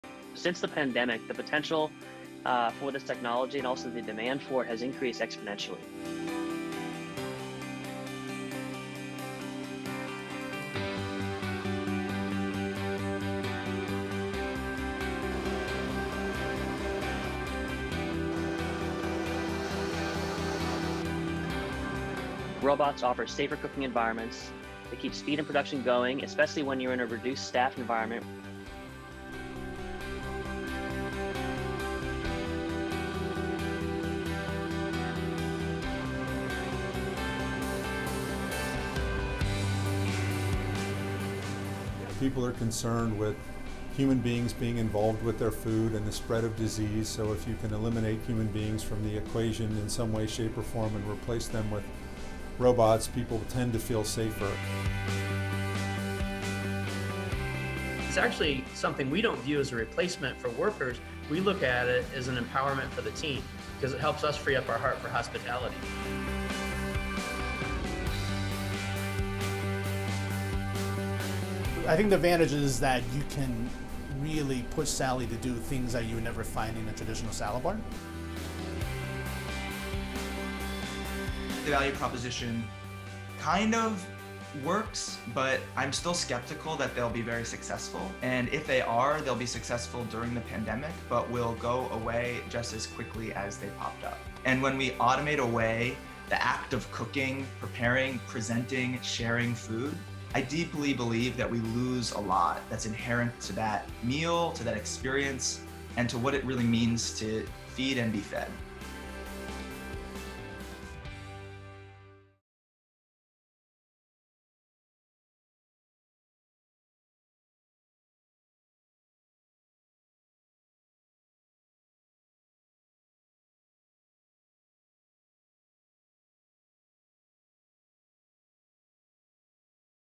Coronavirus Pandemic Feeds Demand for Robot Cooks
The coronavirus pandemic is feeding demand for robots that can flip burgers, make salads and blend smoothies, as businesses and consumers seek to reduce the spread of coronavirus.

Robot food service was a trend before the coronavirus pandemic struck, as hospitals, campus cafeterias and others tried to meet demand for fresh, customized options 24-hours-a-day while keeping labor costs in check.

Now, some say, robots may shift from being a novelty to a necessity.

The U.S. Centers for Disease Control says the risk of getting Covid-19 from handling or consuming food outside the home is low.

Still, there have been numerous outbreaks among restaurant employees and patrons.

"This is the only way you can actually offer a completely contactless and safer for experience to consumers," says Vipin Jain, co-founder and CEO of Blendid, a Silicon Valley startup.

"So, God forbid, next time there is a pandemic, you can maintain the business continuity. You can keep offering food service without shutting down."

Blendid sells a robot kiosk that makes a variety of fresh smoothies.

Customers can order from a smartphone app and tweak the recipe if they want more kale or less ginger, for example.

Once or twice a day, a Blendid employee refills the ingredients.

Only a handful are now operating around San Francisco, but since the pandemic began, Blendid has started contract discussions with hospitals, corporations, shopping malls and groceries.

Starting this fall, the White Castle burger chain will test a robot arm that can cook burgers, French fries and other foods.

The robot, dubbed Flippy, is made by Pasadena, California-based Miso Robotics.

"Since the pandemic, the potential for this technology and also the demand for it has increased exponentially," says Buck Jordan, CEO and co-founder of Miso Robotics.

"Robots offer safer cooking environments. They keep speed of production going, especially when you're in a reduced staff environment."

Jordan says fast food restaurants are already having trouble finding workers, due in part to a shrinking population of young workers.

Flippy currently costs $30,000, with a $1,500 monthly service fee.

By the middle of next year, Miso hopes to offer the robot for free, but charge a higher monthly fee.

White Castle and Miso have been discussing a partnership for about a year.

When Covid-19 struck, those talks accelerated, White Castle vice president Jamie Richardson says.

Richardson says the robot can free up employees for other tasks, like disinfecting tables or handling the rising number of delivery orders.

A touch-free environment that minimizes contact is also increasingly important to customers, he says.

"It's actually something we don't view as a replacement for workers. We look at it as an empowerment for the team, because it helps us free up our heart for hospitality," he says.

As salad bars shut down, California-based Chowbotics started getting more inquiries about Sally, a robot about the size of a refrigerator that makes a variety of salads and bowls.

Sally lets customers choose from 22 prepared ingredients stored inside the machine.

It can make around 65 bowls a day before kitchen workers need to refill the ingredients.

Prior to this year, Chowbotics had sold around 125 of its $35,000 robots, primarily to hospitals and colleges. But since the coronavirus hit, sales have jumped more than 60 percent, claims CEO Rick Wilmer.

Hospital and college demand dropped, but Chowbotics is hearing from grocery stores, senior living communities and even the U.S. Department of Defense, he says.

"It's food automation, and it's definitely coming," says Wilmer.

"And Covid's going to accelerate it again because people are concerned with human beings being involved with their food and the spread of disease."

Wilmer says Sally may actually create jobs, since it keeps selling food at times of day when it wouldn't have been available before.

Max Elder, research director of the Food Futures Lab at the Palo Alto, California-based Institute for the Future, is skeptical about the future of food prep robots once the pandemic has eased.

He also believes the consumers' appetite for food automation is limited.

"When we automate away the act of cooking, preparing, presenting, sharing food, I deeply believe that we lose a lot that's inherent to that meal, to that experience," he says.